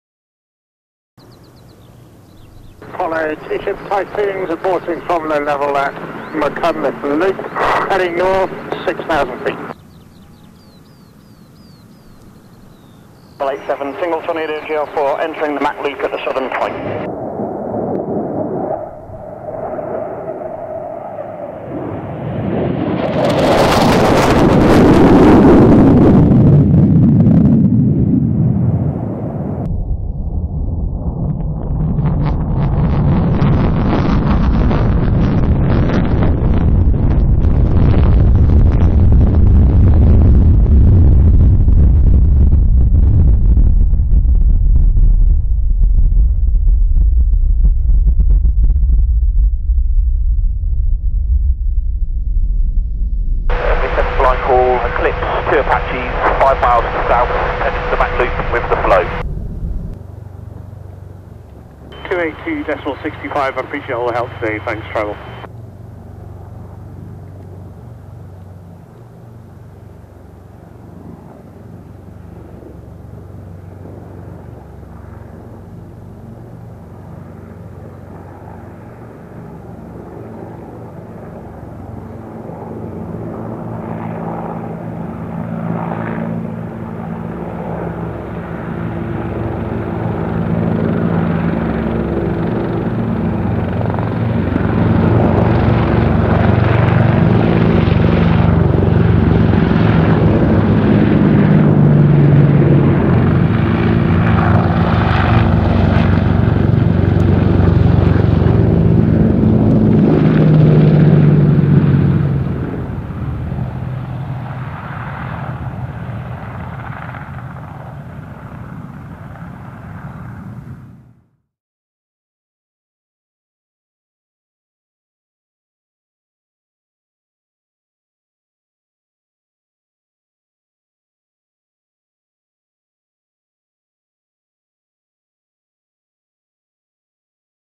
Low Level Flying Mach Loop 2015 P1
Part 1 of my new video for 2015 filmed from Cad East In the Mach Loop LFA7 Wales.  With GR4 Tornado, Apache Helicopter, With Radio Comms